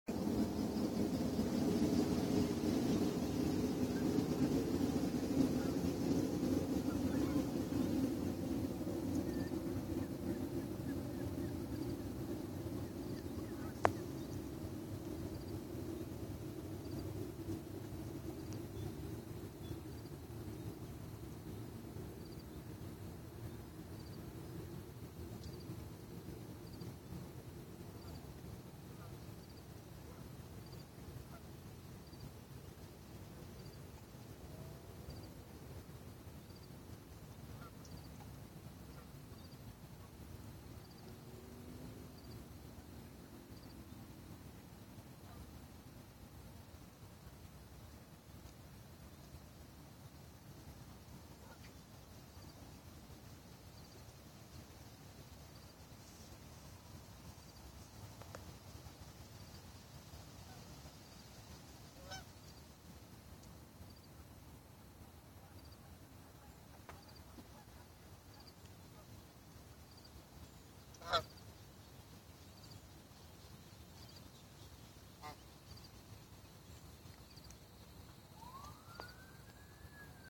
Morning Migration of Birds
The GTA's shoreline was saturated with flocks of birds this morning...thousands of birds flew by me within a time span of 30 minutes.  Some even flew overhead and I felt the air moving with the power of their wings.  What an enhancement to an already magnificent sunrise.  I captured a minute or so here to share with you all.